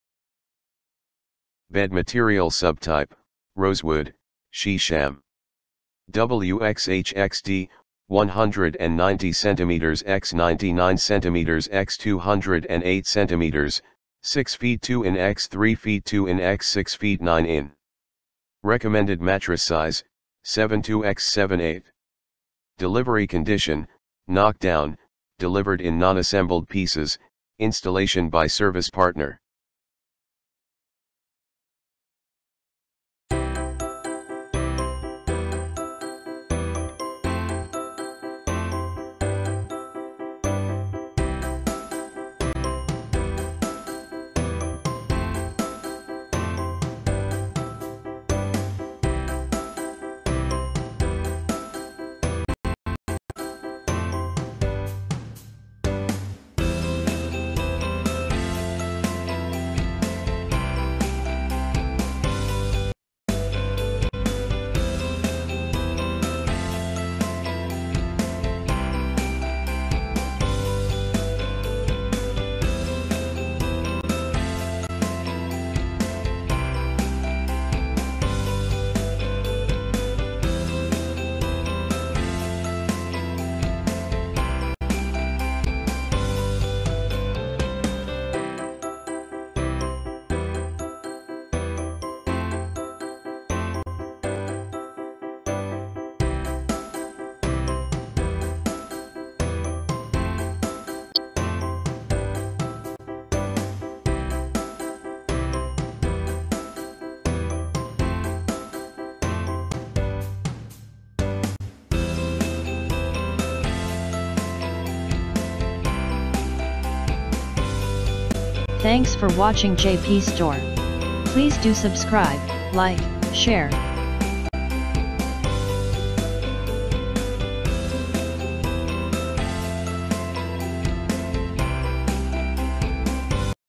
Saffron Art Sheesham Wood Solid Wood King Bed Finish Color Walnut
Bed Material Subtype: Rosewood (Sheesham)
W x H x D: 190 cm x 99 cm x 208 cm (6 ft 2 in x 3 ft 2 in x 6 ft 9 in)
Recommended Mattress Size: 72x78
Delivery Condition: Knock Down - Delivered in non-assembled pieces, installation by service partner